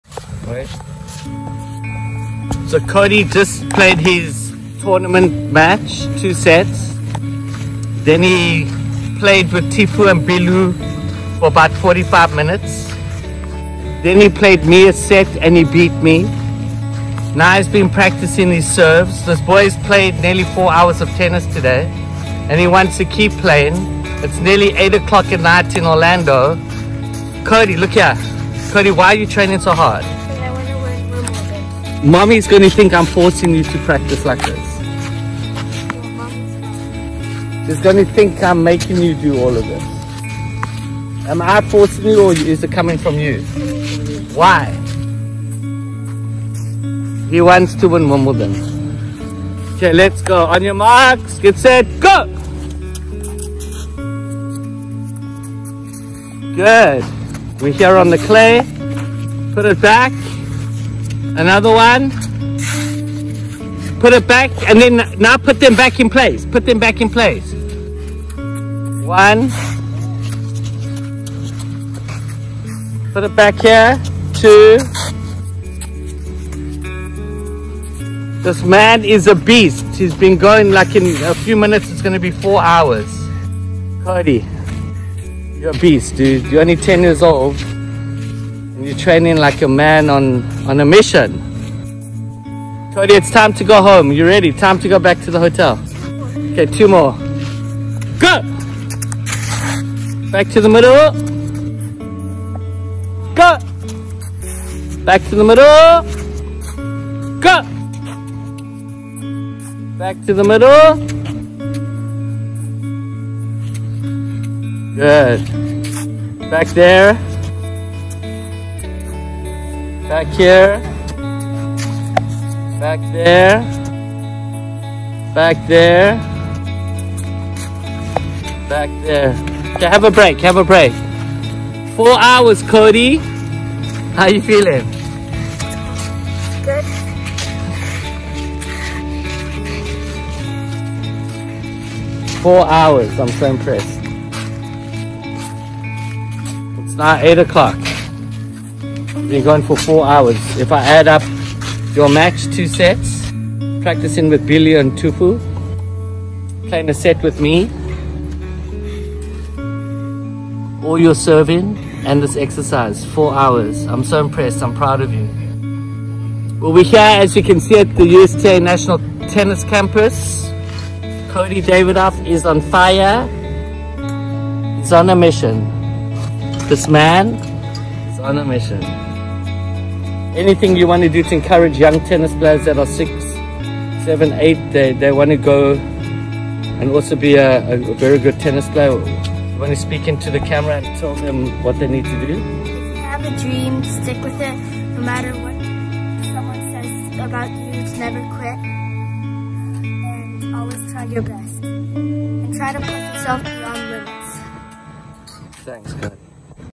4 hours Tennis @ The USTA National Tennis Center in Orlando
4 hours today. Level 1 tennis match on clay @ the National Court Championships, practice with @tennisbehl @tennistif, one set with @jarroddavidoff , serving practice and court sprints